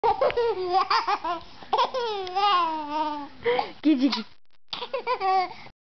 jackson laughing
momma making jackson laugh